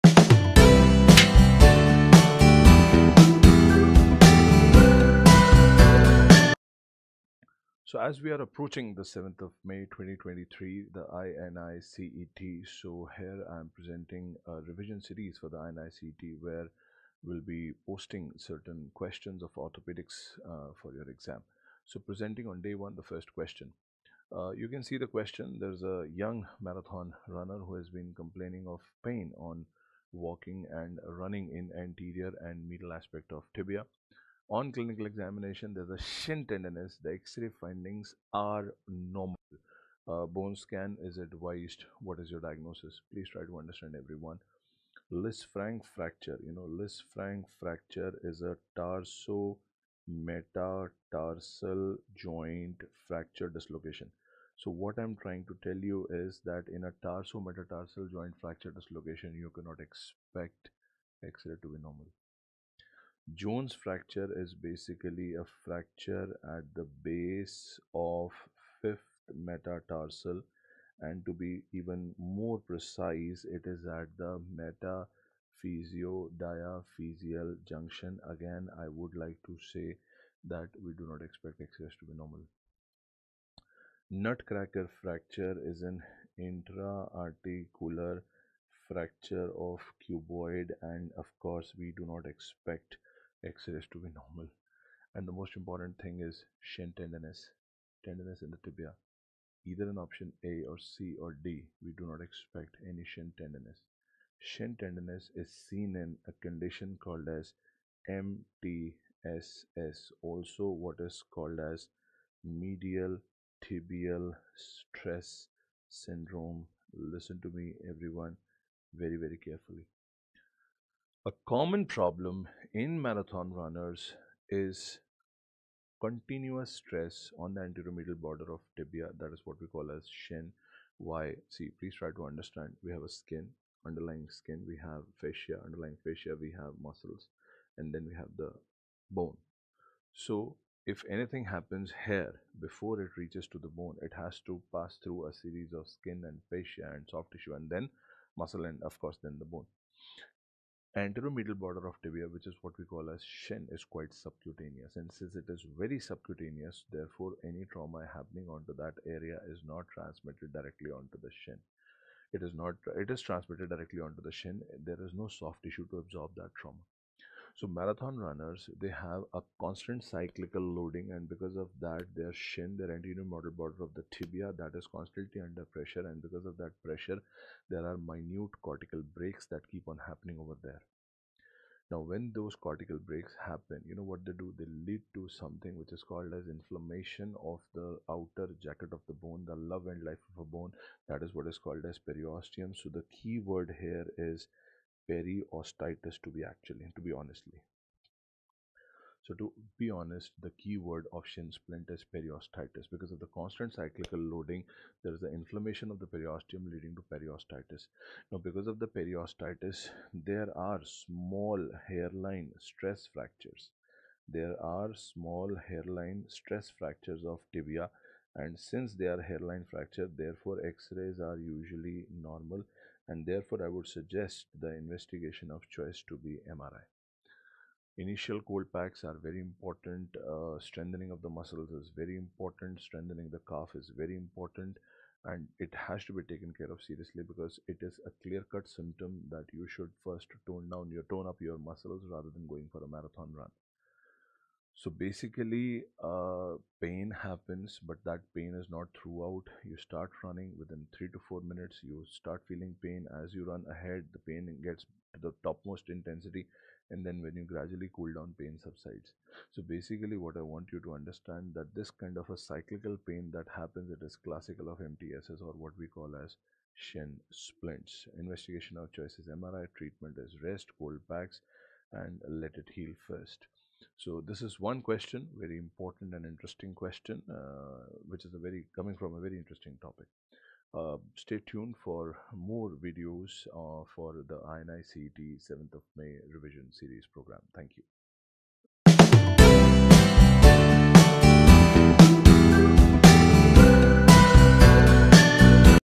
Medial Tibial Stress Syndrome | @DrTusharMehta
This video is an MCQ about Medial Tibial Stress Syndrome / Shin Splints by Dr Tushar Mehta.